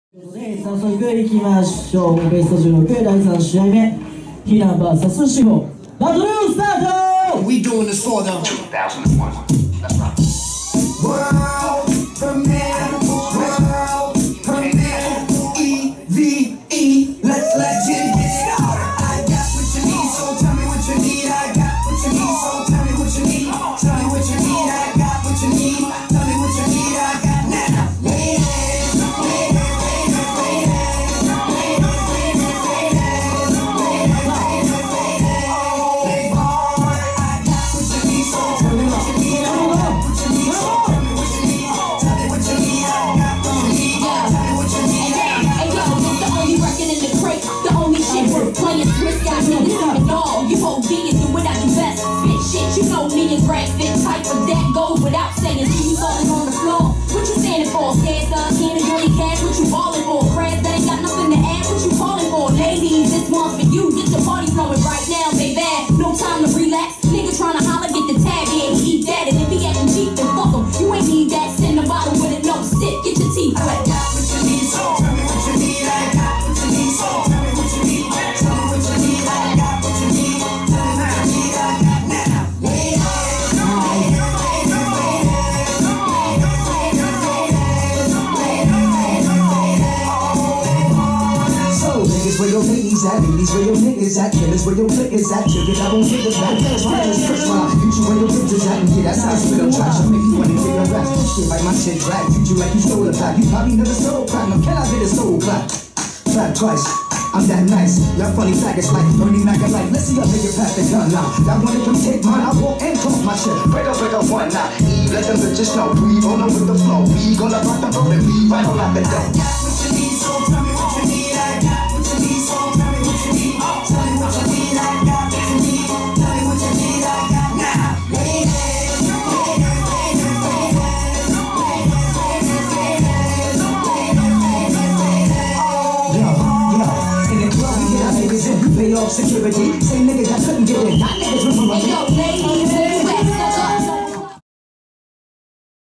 HOTSHOT!!vol.35 女子限定ソロバトル / 16-3 ひな vs 稚歩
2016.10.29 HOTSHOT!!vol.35
女子限定ソロバトル
ベスト16ー3 / ひな vs 稚歩
winner / ひな